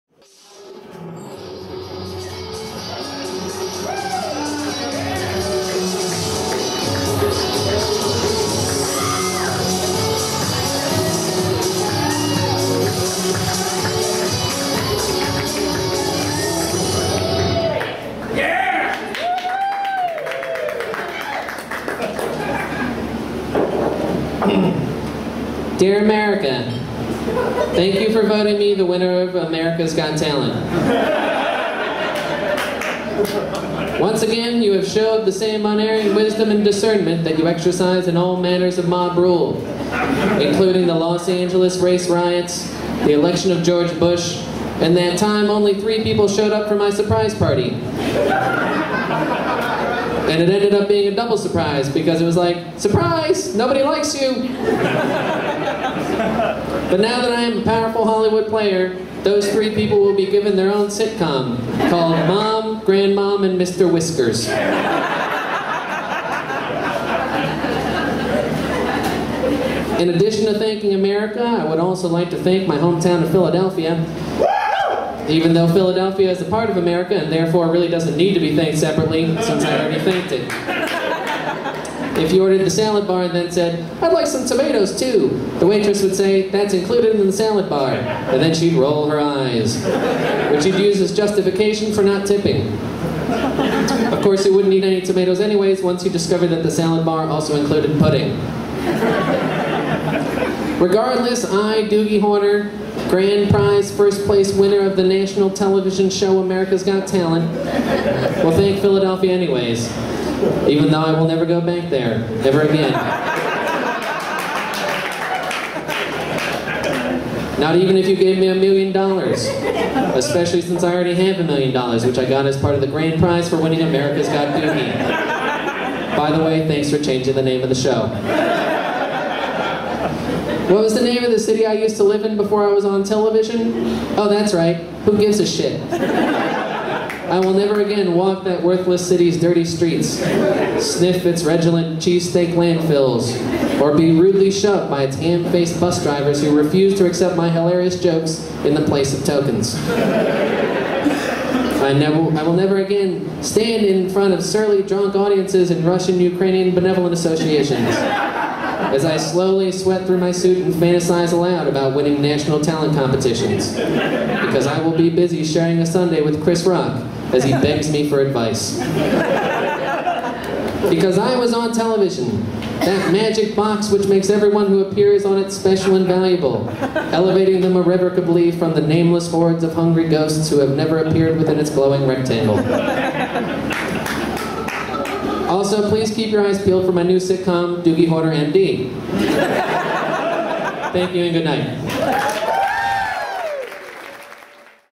Doogie Horner's Letter to America
A thank you note to America, read at the Ministry of Secret Jokes.